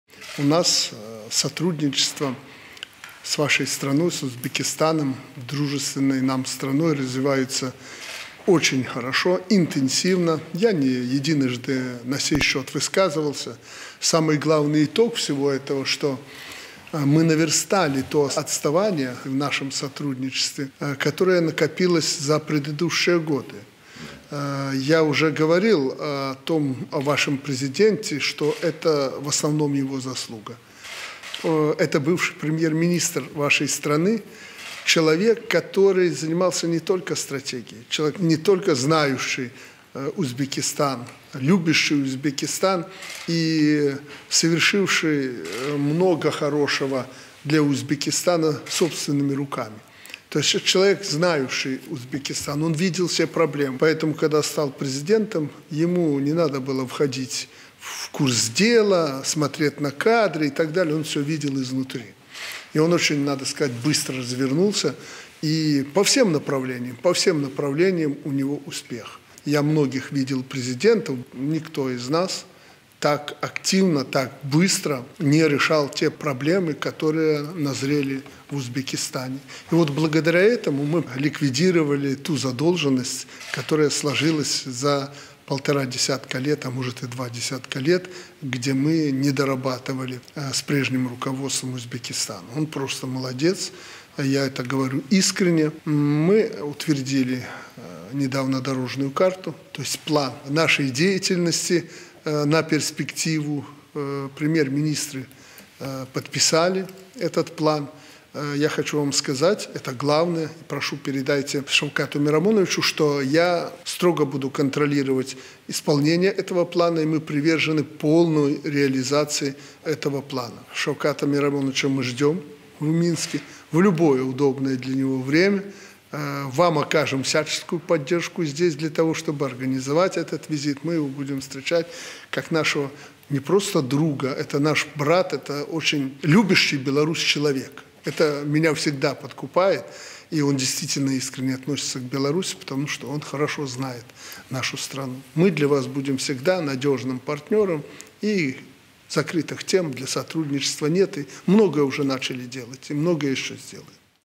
Александр Лукашенко встретился с послом Узбекистана Насирджаном Юсуповым
Президент Беларуси Александр Лукашенко сегодня на встрече с послом Узбекистана Насирджаном Юсуповым подтвердил приверженность страны полной реализации дорожной карты двустороннего сотрудничества.